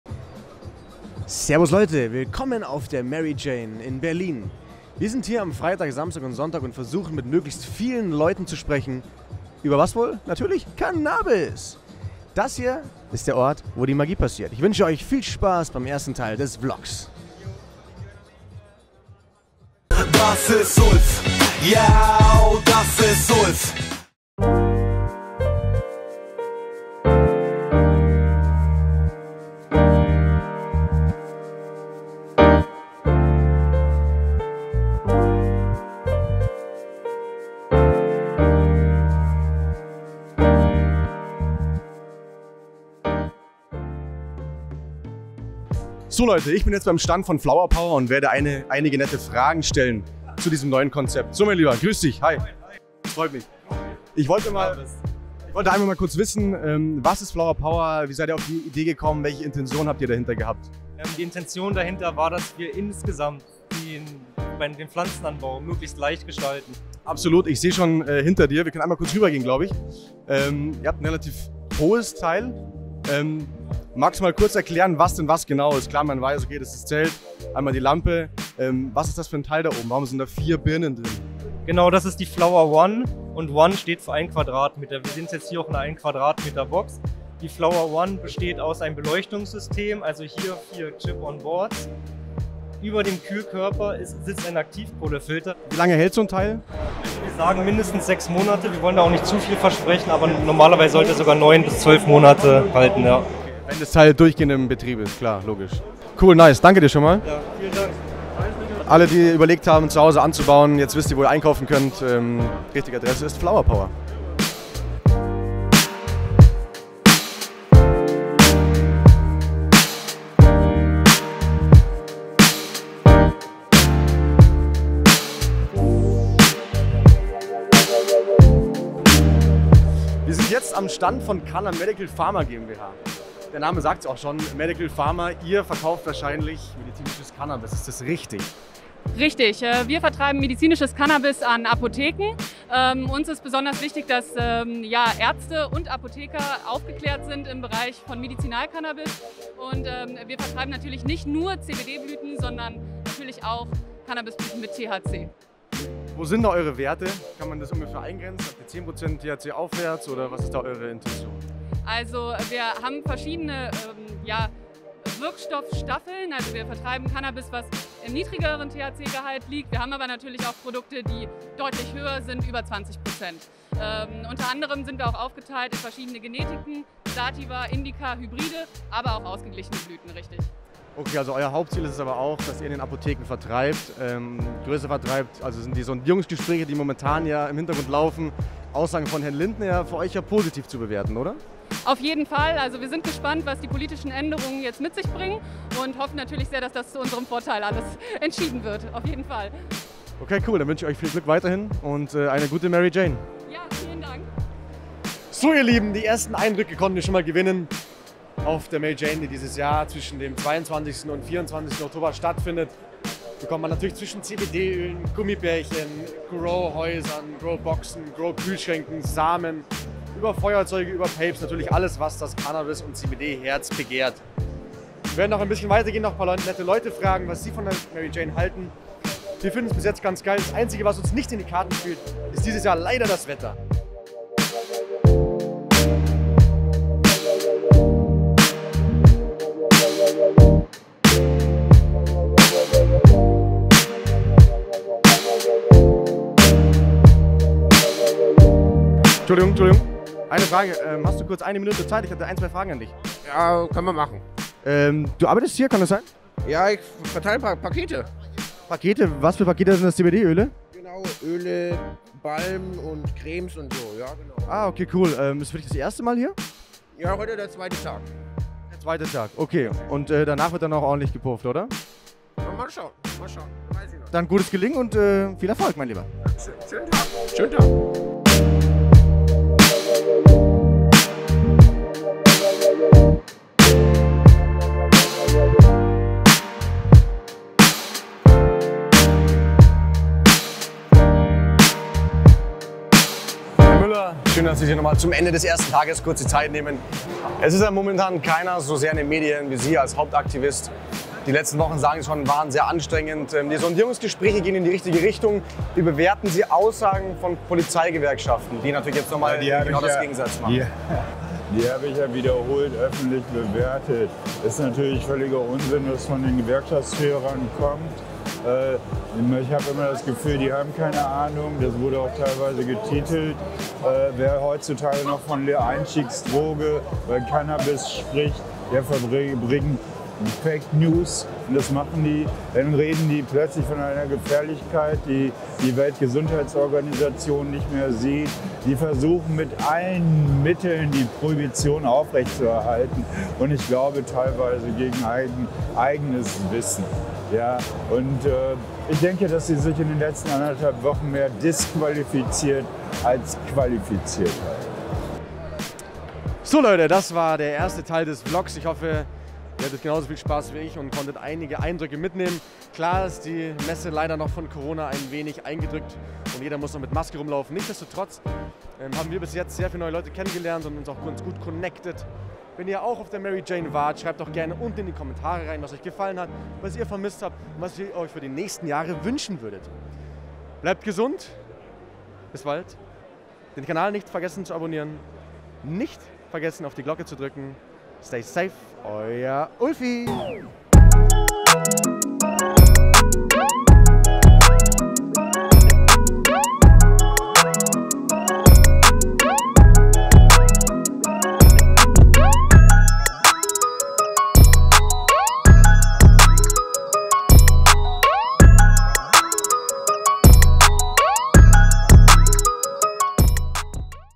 Mary Jane-VLOG mit Jill Hardener, Richter Müller und Georg Wurth in Berlin I Weedo TV I Teil 1
Auf der Berliner Cannabismesse tummelten sich mal wieder mehrere zehntausend Besucher und über 35.000 verschiedene Produkte konnte man bestaunen. Die Mary Jane fiel dieses Jahr aufgrund von Corona in den Oktober, was viele Hanffreunde nicht davon abhielt sie zu besuchen. Die Stimmung war so gut wie noch nie, da momentan im Hintergrund ja die Sondierungsgespräche laufen und einiges auf eine Cannabislegalisierung hindeutet. Die Anziehungskraft der Mary Jane ist enorm, nicht nur der interessierte junge Hanffreund aus dem Berliner Vorort ist anwesend. Auch Stars und Sternchen aus anderen Branchen nutzen die Mary Jane als Präsentationsfläche entweder für sich selbst als Bühne oder als Werbung für die eigenen Produkte. Neben Marvin Game, Plusmacher, Richter Müller oder Georg Wurth vom DHV waren unter anderem auch SSIO, Jill Hardener, Maximilian Pollux oder Herzog zu sehen. Die Interviews mit Jill Hardener und Georg Wurth kommen dann im zweiten Teil des VLOG´s. Da das Wetter am Freitag nicht so mitspielte, war der Außenbereich am Samstag prall gefüllt und auch die Live-Acts machten Stimmung ohne Ende. Am Sonntag wurde es dann ein wenig gediegener. Es konnte entspannt zusammen getrunken oder gegessen werden und viele Stände wurden im Laufe des Nachmittags abgebaut. Ob man nächstes Jahr auf der Mary Jane dann schon reine THC-Produkte bestaunen kann bleibt abzuwarten. Die Chancen stehen nicht schlecht.

Euer Ulf reist für euch durch ganz Deutschland und interviewt Promis zum Thema Legalisierung von Cannabis.